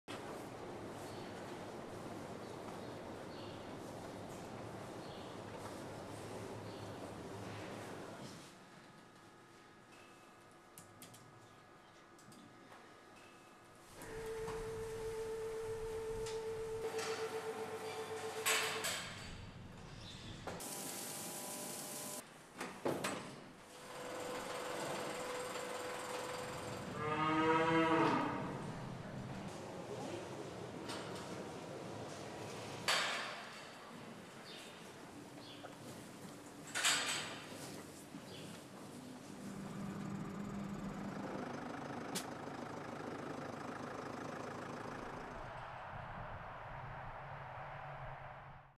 GEA Dairy Farming - GEA MixFeeder mit WIC im Überblick
Unser MixFeeder mit WIC im Überblick
• Präzise und individuelle Gruppenfütterung
• Gesunde und leistungsfähige Kühe für einen größtmöglichen Milchertrag
• Flexibles Arbeitsumfeld mit effektiven Arbeitsabläufen
• Einfaches und effizientes Futtermanagement
• Höchstes Informationslevel durch Live-Display und Meldungen für das Smartphone
• Intelligente Berechnung der Fütterungsrouten